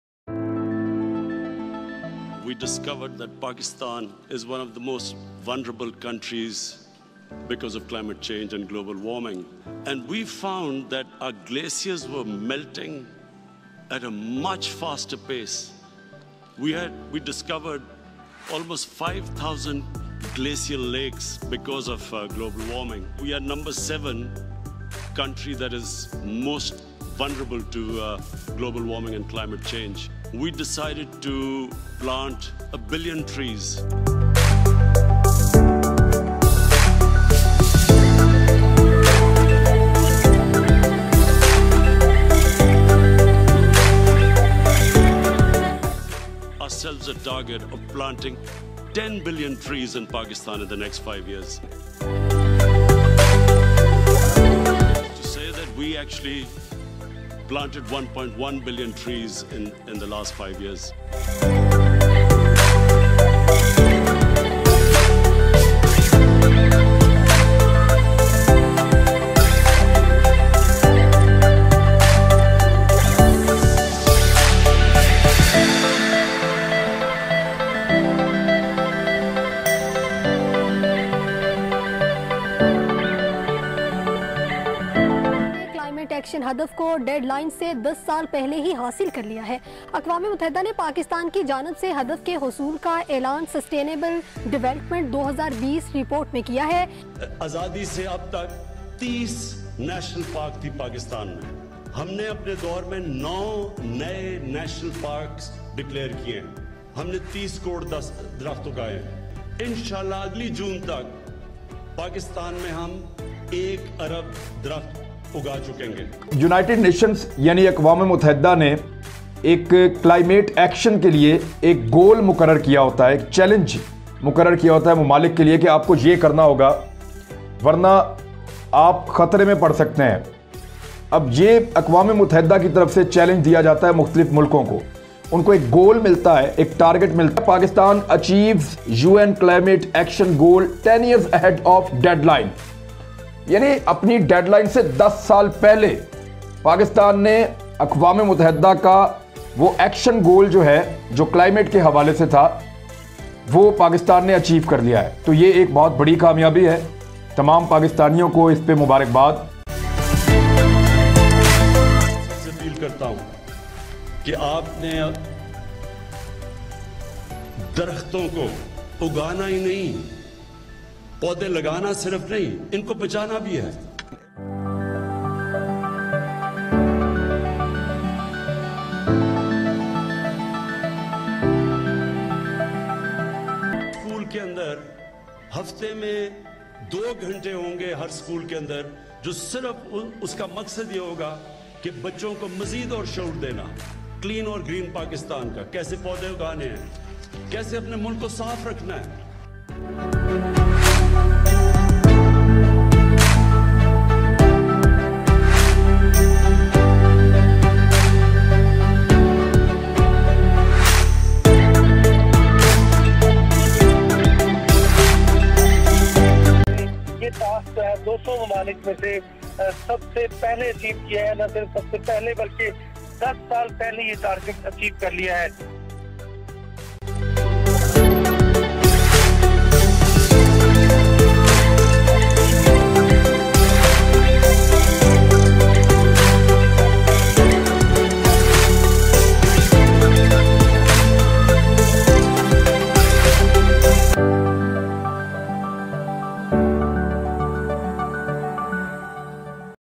Pakistan achieves UN climate action goal 10 years ahead of deadline
Pakistan has passed a crucial milestone by achieving an overarching Climate Action Sustainable Development Goal (SDG-13) a decade ahead of the 2030 deadline for its implementation, according to the UN’s annual sustainable development report.Advisor to PM on Climate Change Malik Amin Aslam termed it a landmark achievement for the country. Aslam further said that government’s initiatives such as the 10 Billion Tree Tsunami Programme, Clean Green Pakistan Initiative and Clean Green Pakistan Index played a vital road in the achievement.